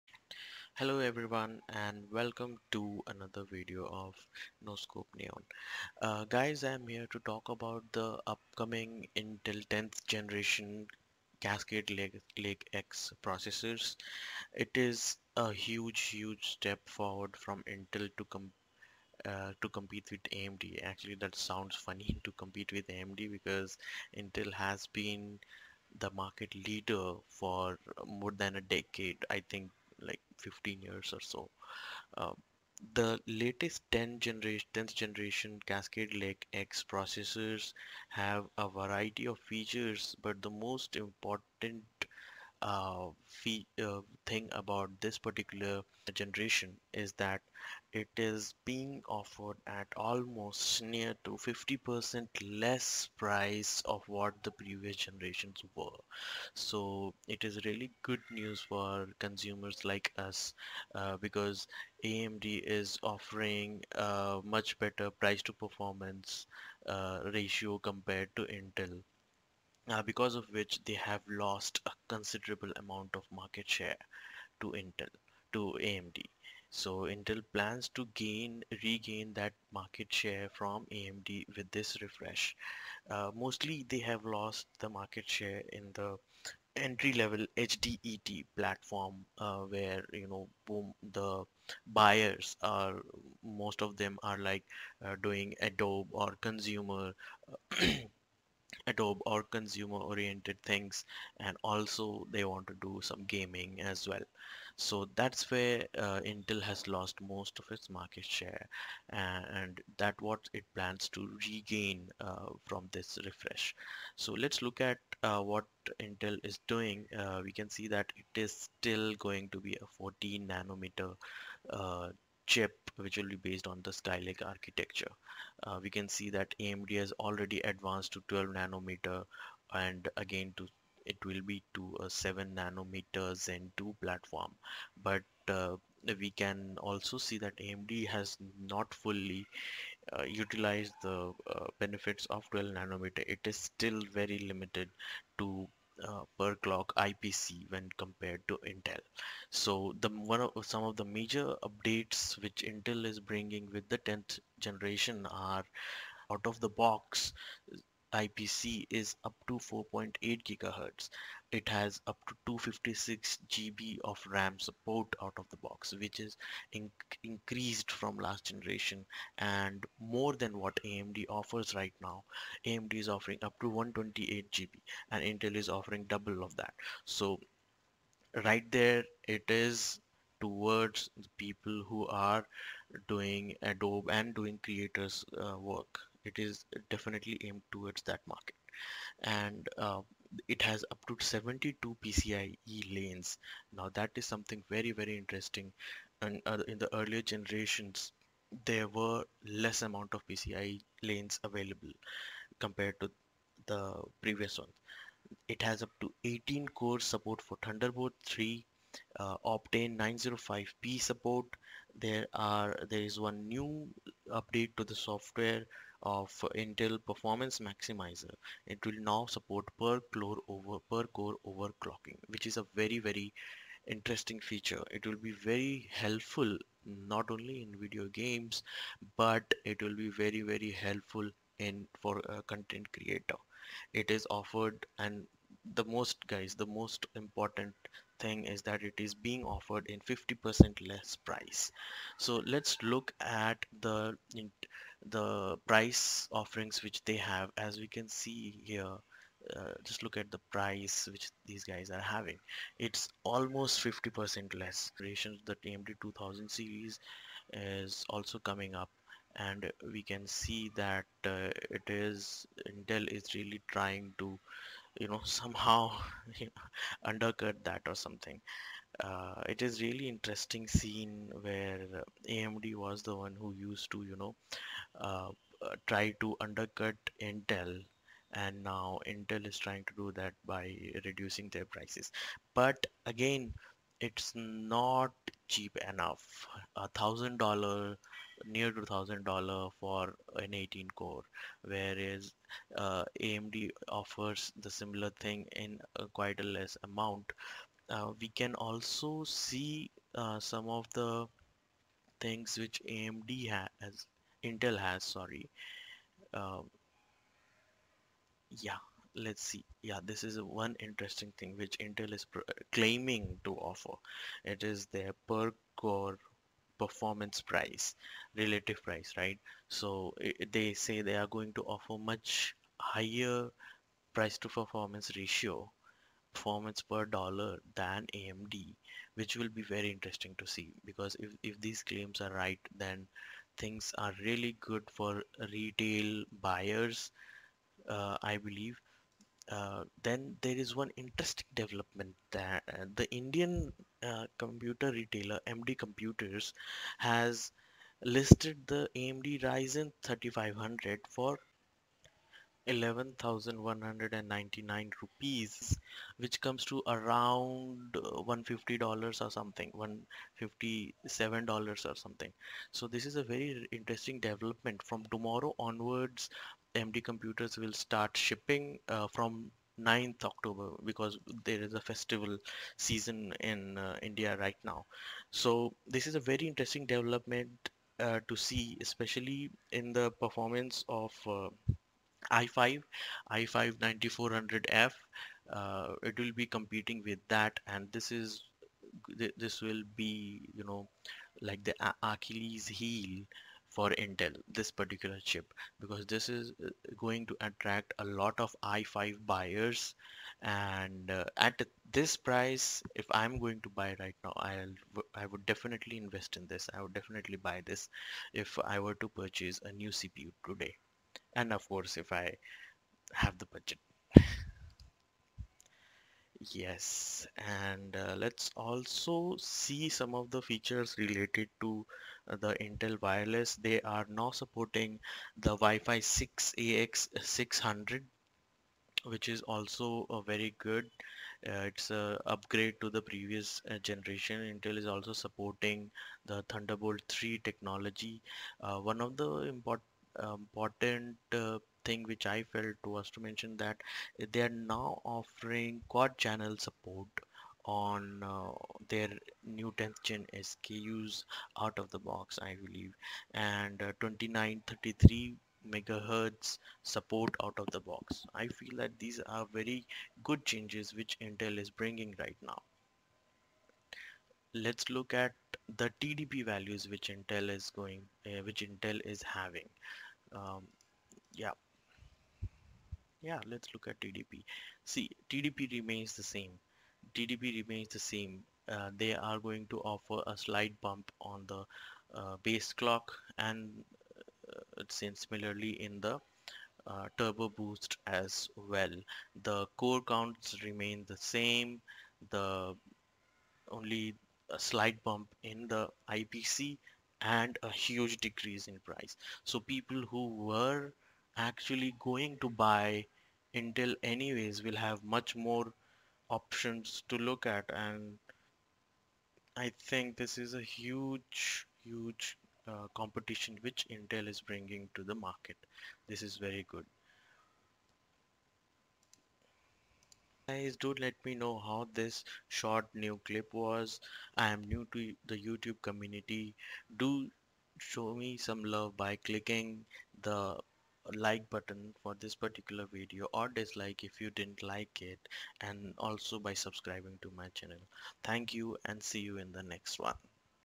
Intel 10th gen cascade lake X price cut | Should you buy? | AMD Ryzen 5 3500
Hi guys, Here is my first take on the upcoming Intel 10th generation cascade lake X series processors. Intel has announced 50% less price compared to previous generations which was an interesting development.

Also, AMD Ryzen 5 3500 towards the end of the video. Opinions are welcome